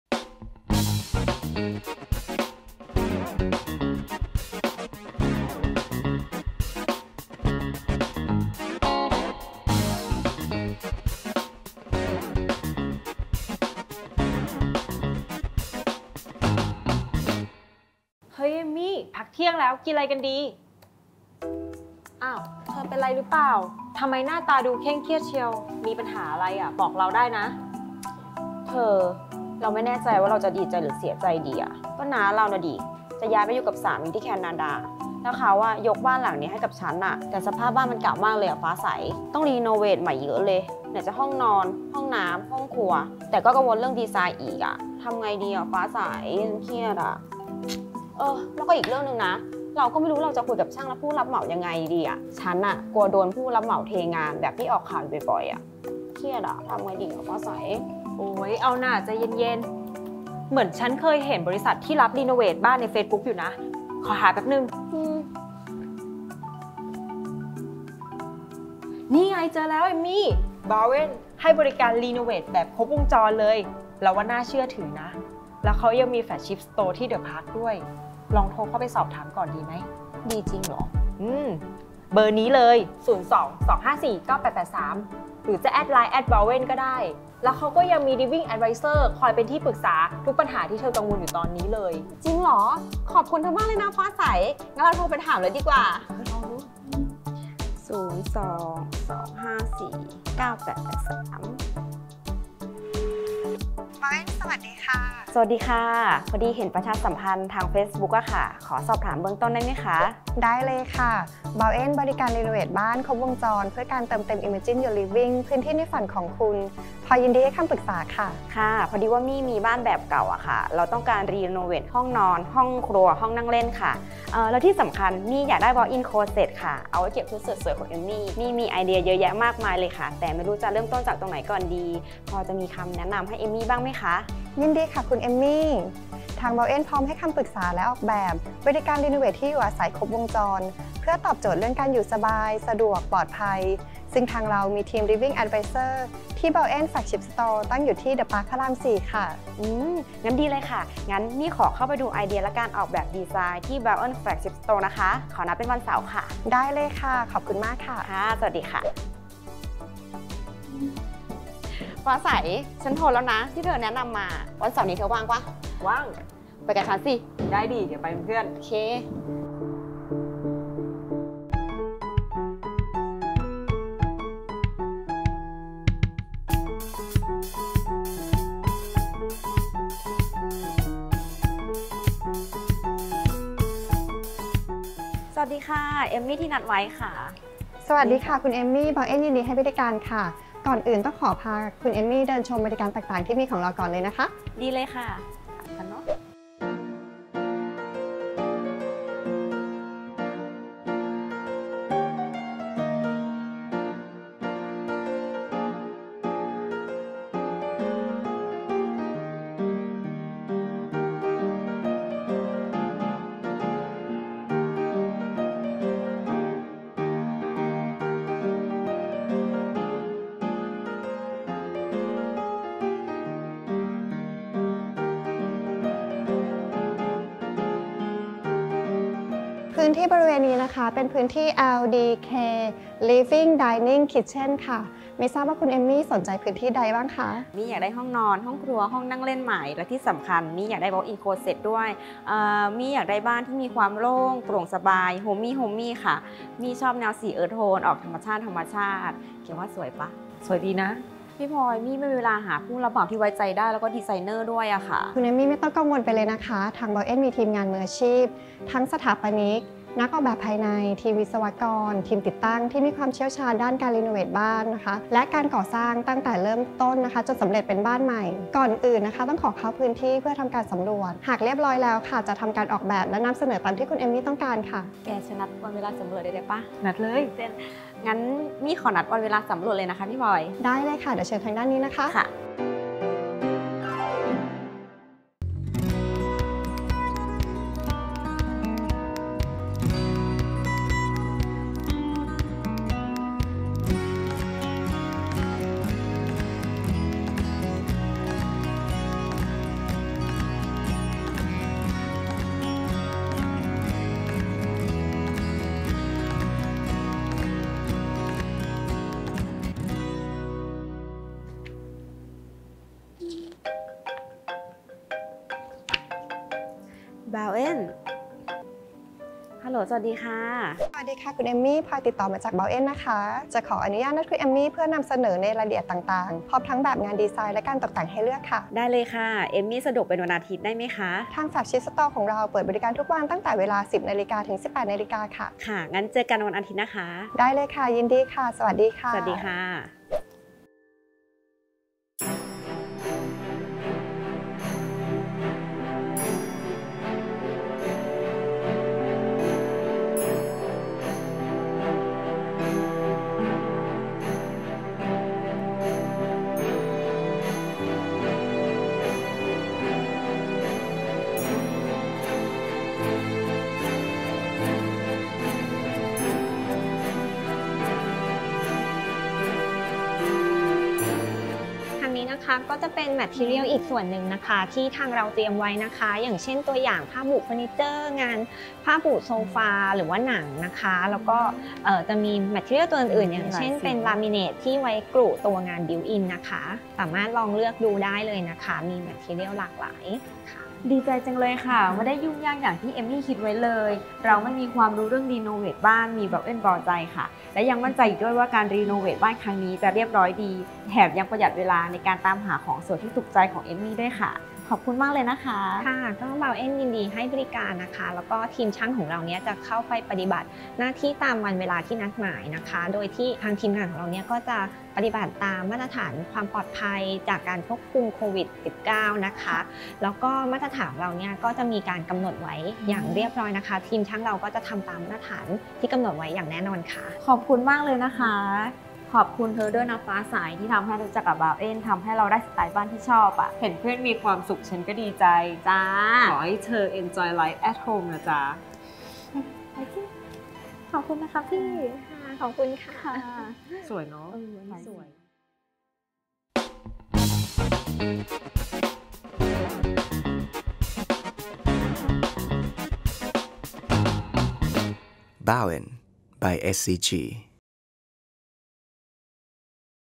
BAUEN Flagship Store ตอบโจทย์ความต้องการครบครันเรื่องรีโนเวตบ้าน
ใครมีแพลนรีโนเวตบ้านสามารถเข้ามาปรึกษา BAUEN Flagship Store   พร้อมดูไอเดียการออกแบบบ้านแบบครบวงจร และปรึกษาผู้เชี่ยวชาญเพื่อให้ได้แบบที่ตรงใจมากที่สุด บอกเลยว่าตอบโจทย์ความต้องการอย่างครบครัน ช่วยเติมเต็ม 'Imagine Your Living' พื้นที่ในฝันของคุณ

รวมถึงมีทีมงานมืออาชีพ สถาปนิก นักออกแบบภายใน ทีมวิศวกร ทีมติดตั้ง ที่มีประสบการณ์เชี่ยวชาญเรื่องการรีโนเวตบ้านและการก่อสร้างตลอดทั้งกระบวนการจนสำเร็จเป็นบ้านใหม่ดั่งฝัน และยังมีห้องจำลองบ้านตัวอย่าง ( Interior Showcase )โซนจัดแสดงห้องจำลองภายในบ้านที่แบ่งพื้นที่การใช้งานต่างๆ พร้อมสินค้ามากมายที่จะสร้างแรงบันดาลใจอย่างที่ต้องการ

📍BAUEN Flagship store at The Parq Rama 4 เปิดบริการทุกวัน
⏰ตั้งแต่เวลา10:00-18:00 น.

Line official account : @BAUEN